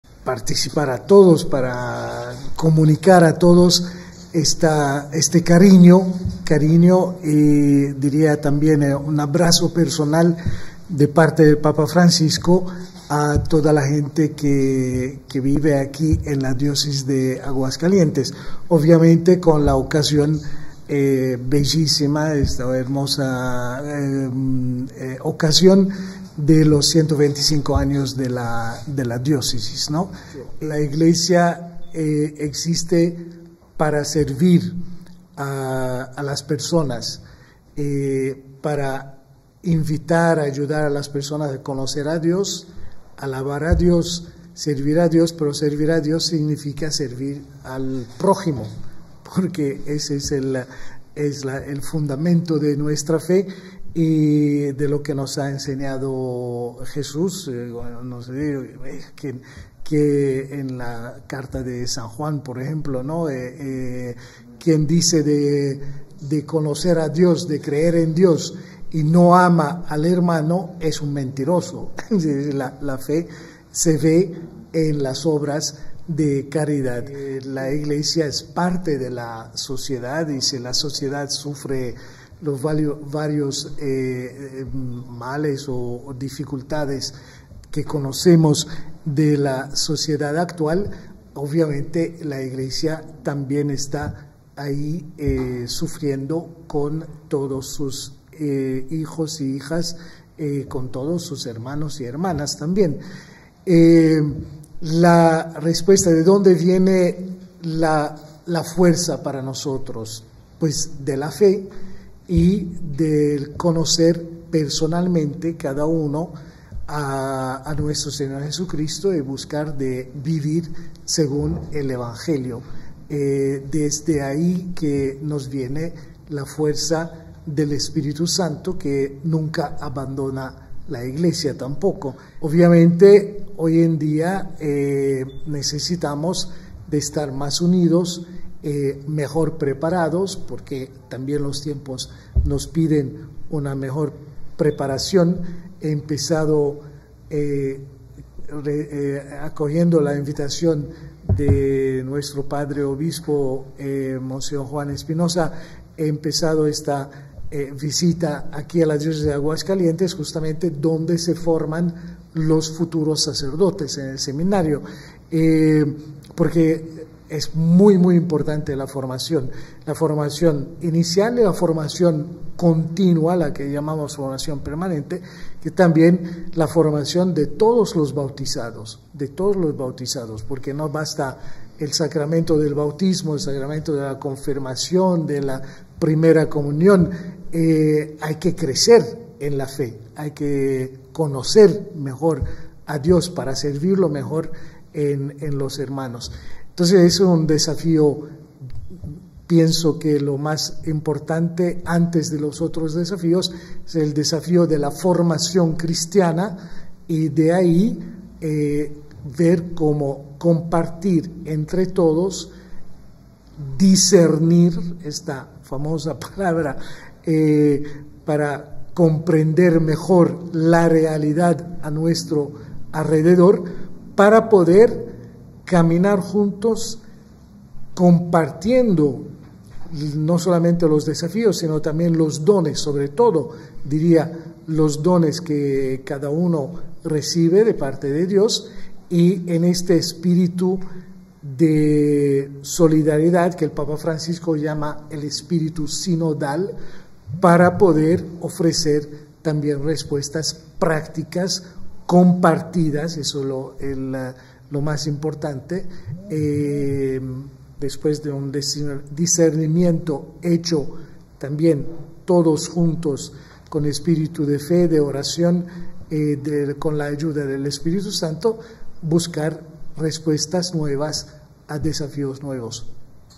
Quien dice creer en Dios y no ama al hermano es un mentiroso: Joseph Spiteri
Servir a Dios significa servir al prójimo, quien dice conocer y creer en Dios y no ama al hermano es un mentiroso: Joseph Spiteri

* La fuerza del Espíritu Santo nunca abandona a la Iglesia y le permite enfrentar los constantes ataques
* Es muy importante la formación constante de todos los bautizados, hay que crecer en la fe, hay que conocer mejor a Dios

Aguascalientes, Ags. 26 de agosto 2024.- Servir a Dios significa servir al prójimo, pues quien dice conocer a Dios y creer en Dios pero no ama a su hermano es un mentiroso, expuso el Nuncio Apostólico en México, Joseph Spiteri.
En breve conferencia de prensa ofrecida con motivo de su presencia en esta Diócesis para encabezar los festejos por el 125 aniversario de su fundación y tras participar a los hidrocálidos del cariño y un abrazo personal del Papa Francisco, el representante papal en nuestro país hizo hincapié en la importancia de mantener la formación permanente de todos los bautizados, ya que es necesario conocer mejor a Dios para servir mejor a nuestros hermanos.
Cuestionado en los constantes ataques en contra de la Iglesia Católica, Joseph Spiteri reconoció que esto ha ocurrido hace 100, 200, mil y dos mil años y siempre se se ha tenido la fuerza del Espíritu Santo, lo que permite enfrentar los constantes ataques de que han sido objeto.